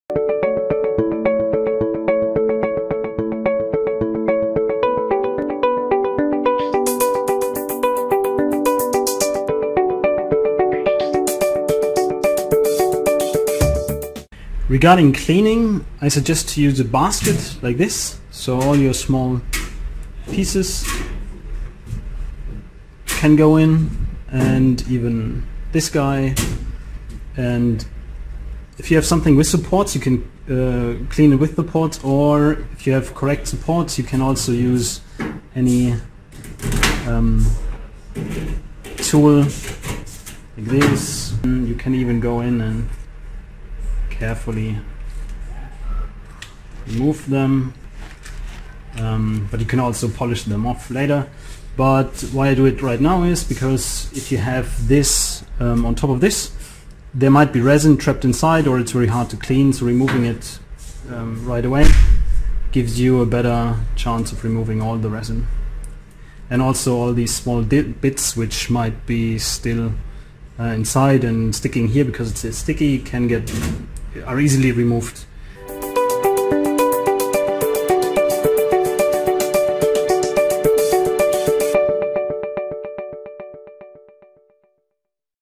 Removing Supports After Printing on Asiga 3D Printers
Learn how to remove the supports after printing on the Asiga 3D Printers.